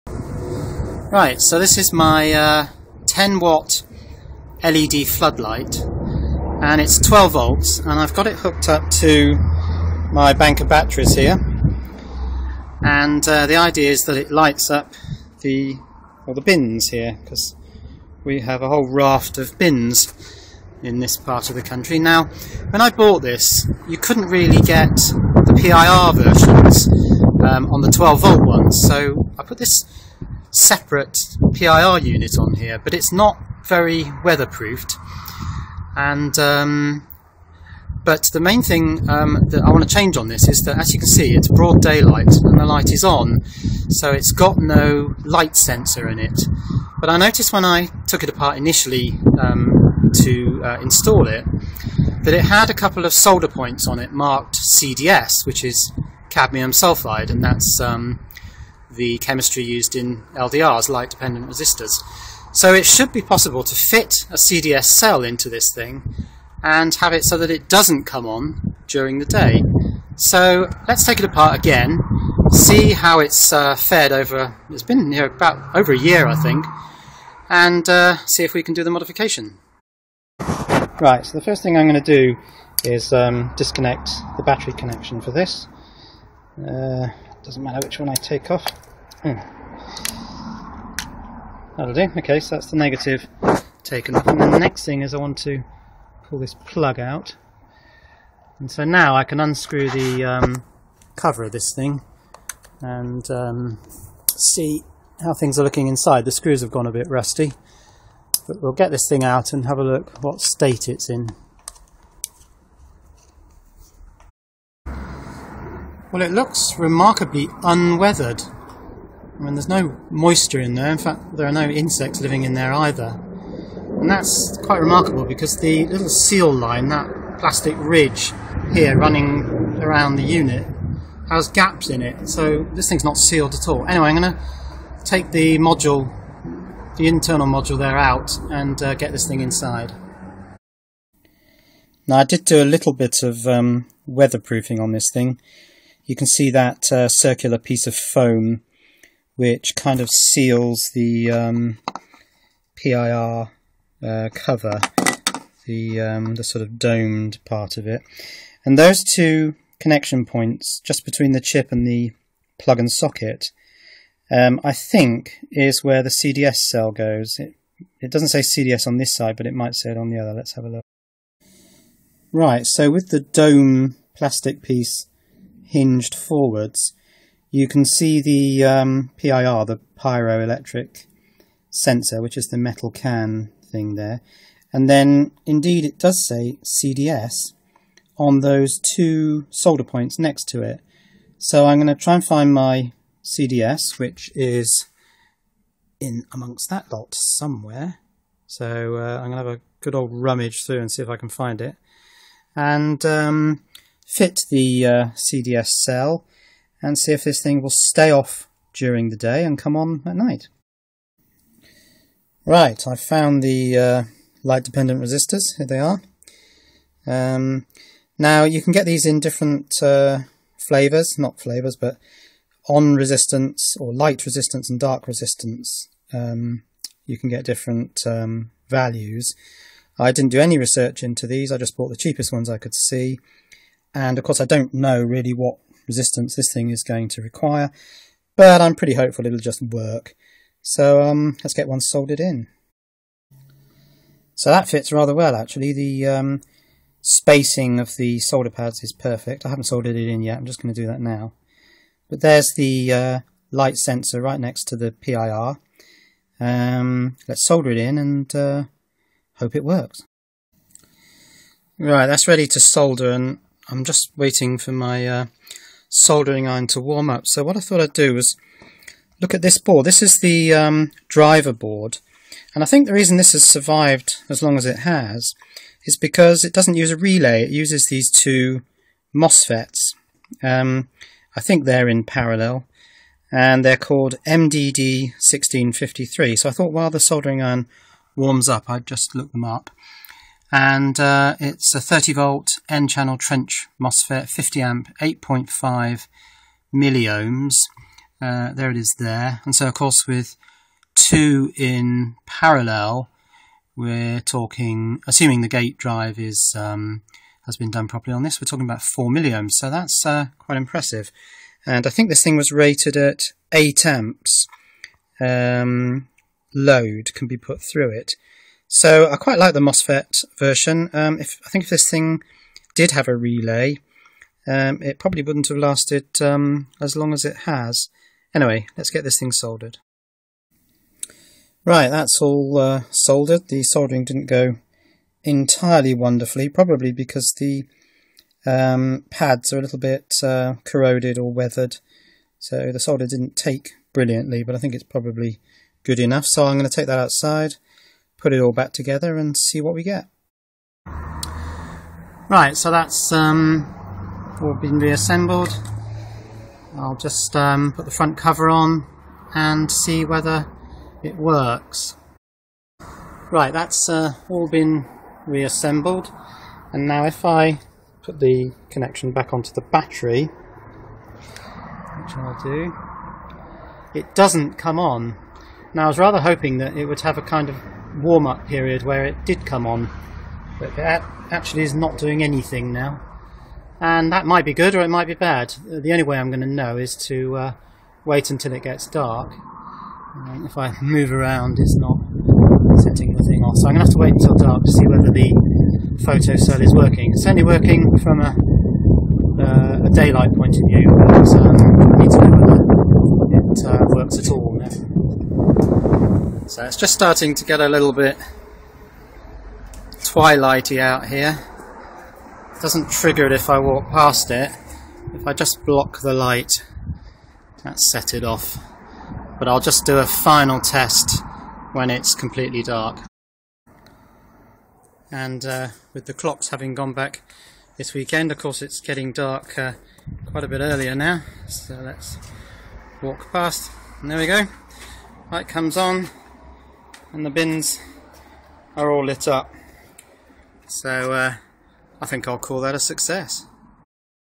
10W LED PIR Floodlight Upgrade - Fitting a CdS Light Dependent Resistor
This neat little 12v PIR sensor has survived a year in the great outdoors, partly because it uses MOSFETs for switching rather than a relay. But it comes supplied without a daylight sensor, so it has always turned on during the day, wasting precious solar generated battery power. Here I fit the missing CdS (cadmium sulphide) light dependent resistor (LDR) to ensure it operates at night only.